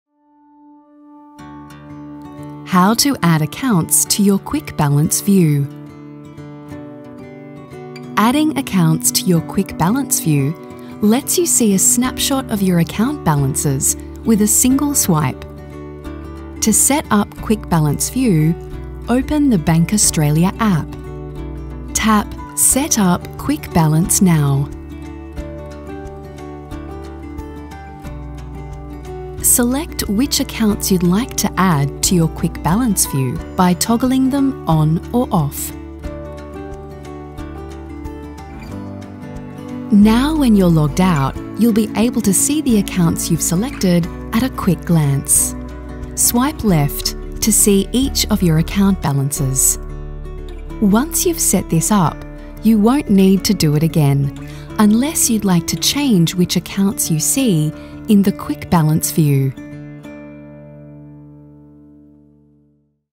How to add accounts to your quick balance view
In this video, we show you step by step how to set up Quick Balance, so you have fast, easy and safe access to your account balance.